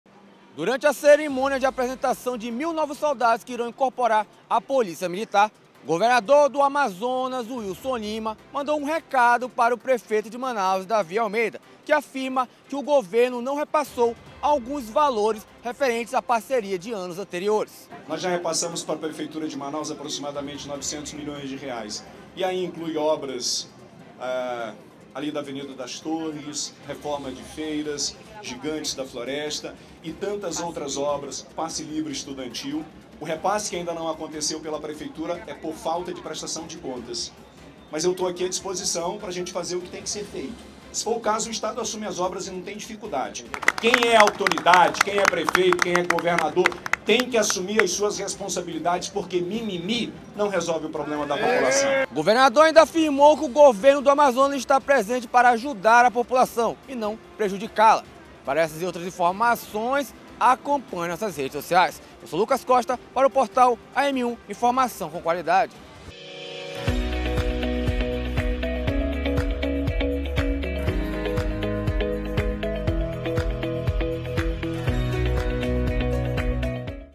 Wilson Lima afirma ter feitos os repasses combinados entre governo e prefeitura
O governador ainda disse que  a prefeitura precisa fazer "a prestação de contas" para o repasse dos valores pendentes em vez de ficar com "mimimi".